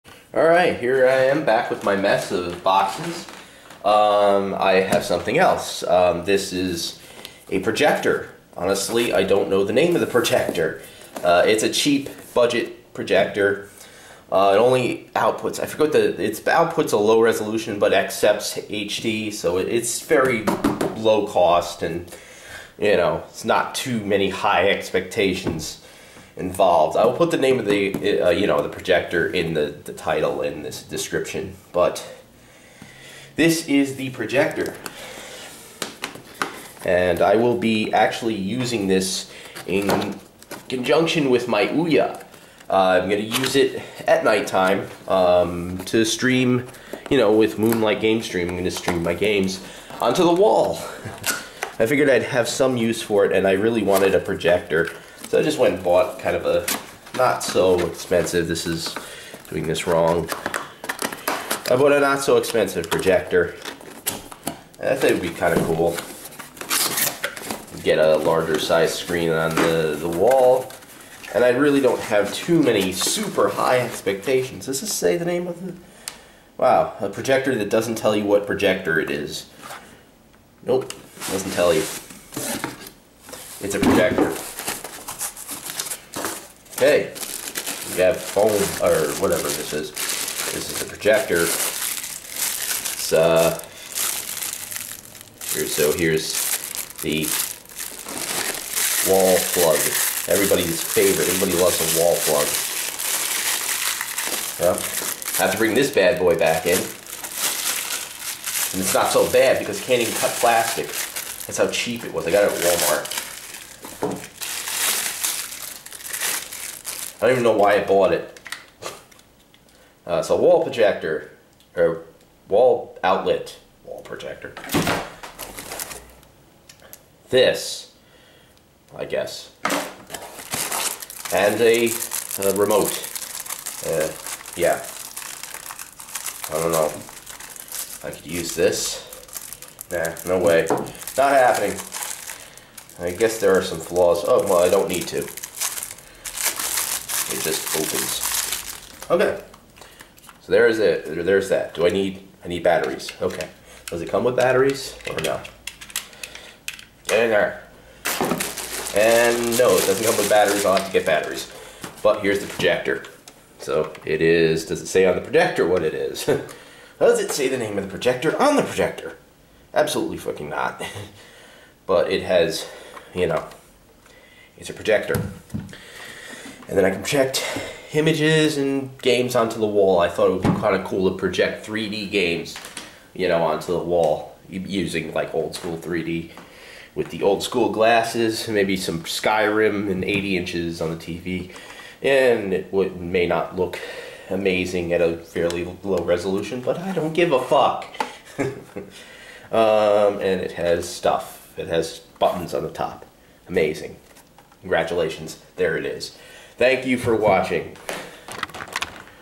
Abdtech portable projector unboxing
A budget projector. It does not output high def. It will take up to a 1080p input but it downscales it to 640x480. So it's the sharpest but it looks good for a budget device.